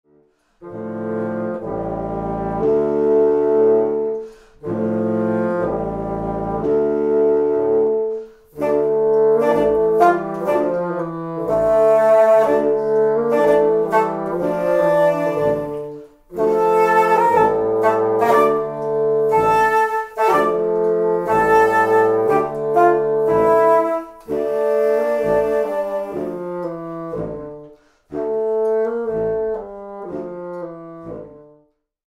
Ludlow Mini Bassoon
Here's a snippet from a recent bassoon quartet video we made - here we've cut to the section where the Ludlow Mini bassoon comes in, to highlight the quality of the sound, and how fun these instruments can be for the younger generations! These little bassoons are perfect for ages 7-11 - learn more about them